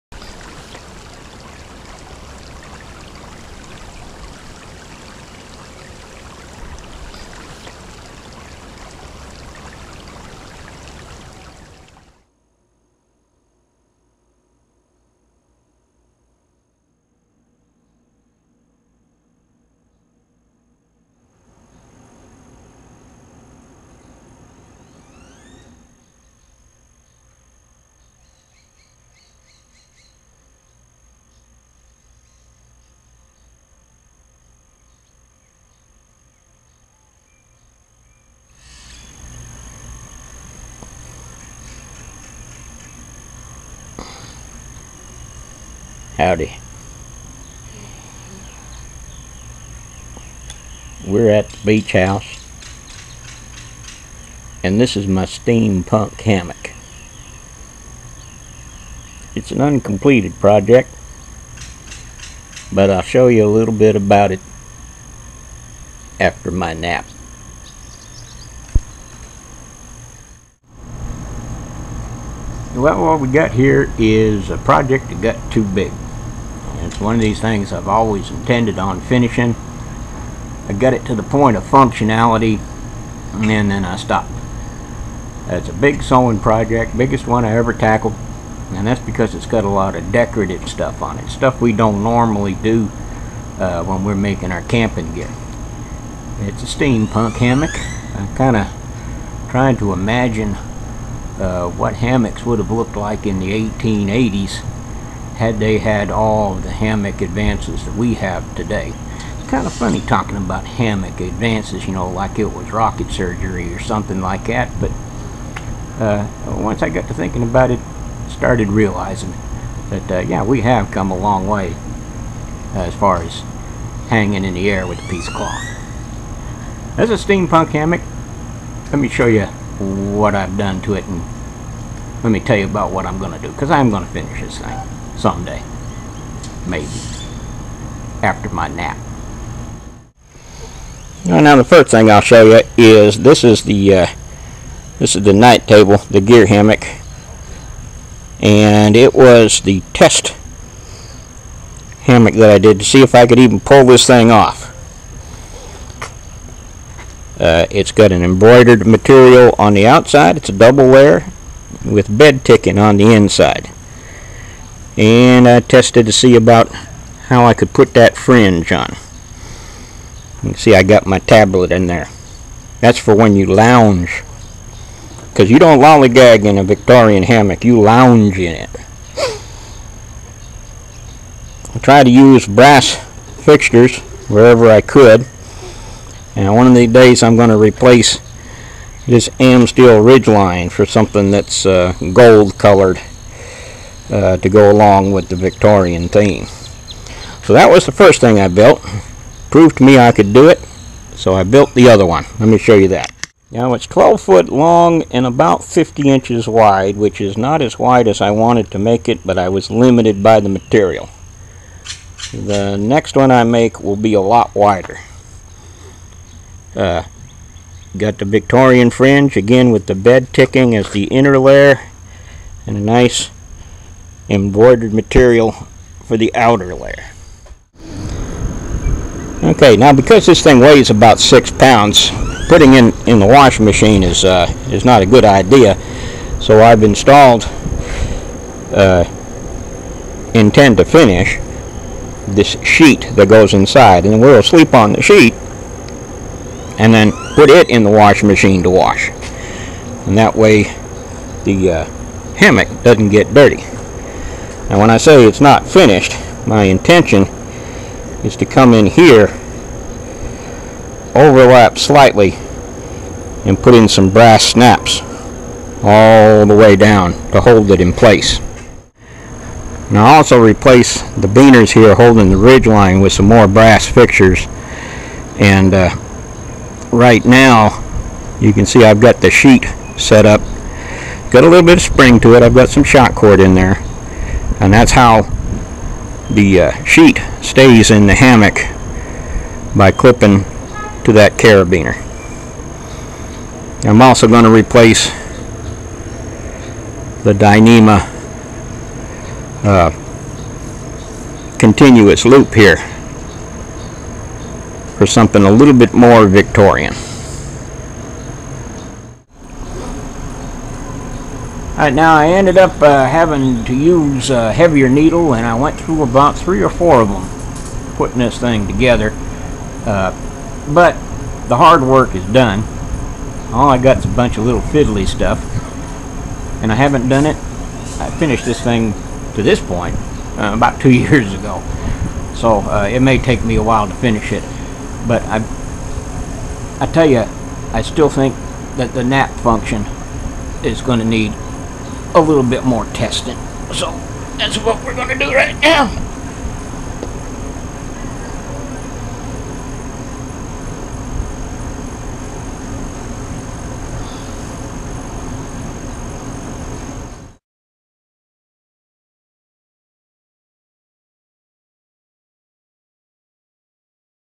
HAMMOCK CAMPING GEAR: Lollygagging In The Steampunk Hammock
The Little Colonel's sister offered us her beach house for the weekend,  so I dragged the old Steampunk Hammock out of mothballs and headed to Galveston for some high brow lollygagging. Its an unfinished project that I really need to finish, and this might give me the gumption I need to get that done.